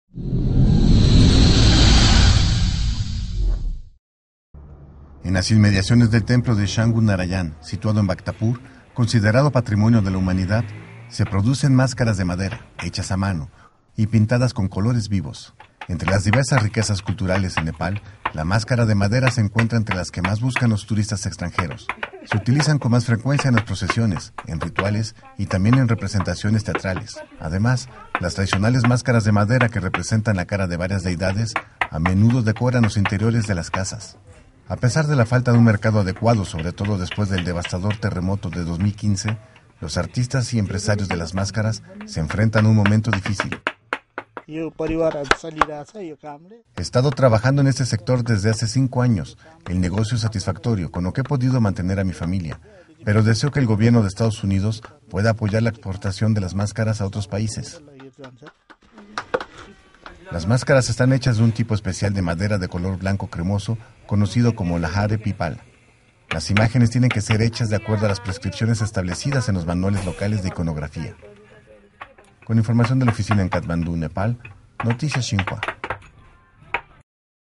Máscaras de madera, un bien cultural en Nepal
En las inmediaciones del templo de Changu Narayan, situado en Bhaktapur, considerado patrimonio de la humanidad, hay un sitio un sitio donde se producen máscaras de madera hechas a mano, cubiertas con barro y ropa de cama y pintadas con colores vivos.